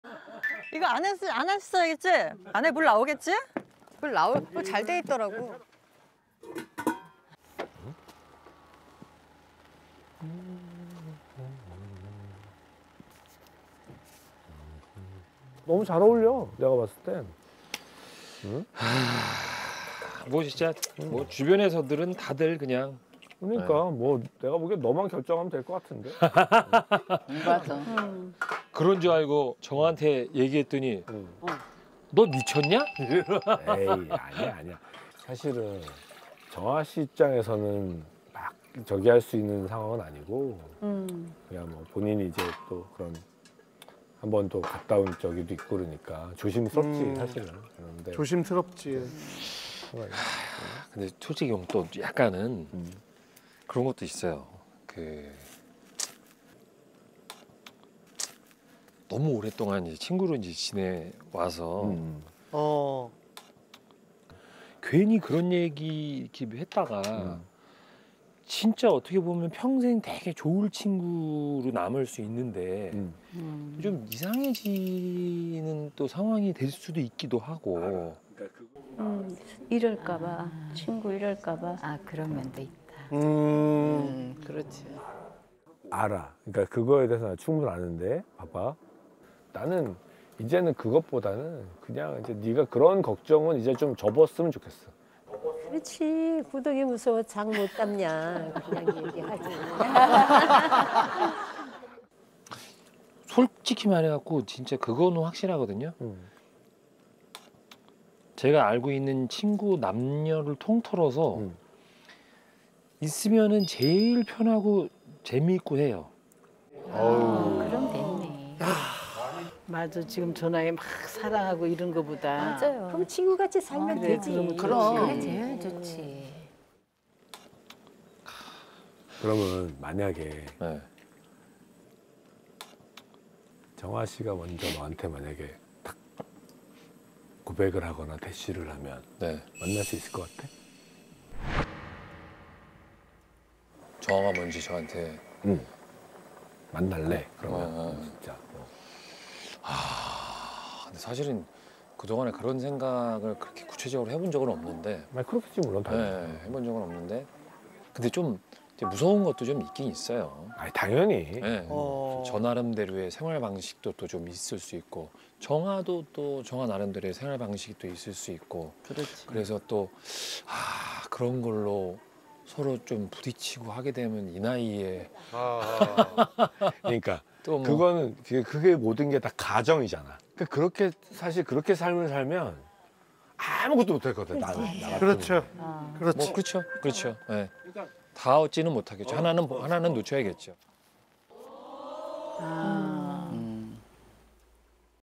“조금 무서운 것도 있어요” 김승수, ‘절친’ 손지창 앞에서 털어놓는 속마음!
김승수는 절친인 손지창에게 양정아에 대한 속마음을 이야기한다.

본방송 | 5월 5일 일요일 밤 9시 5분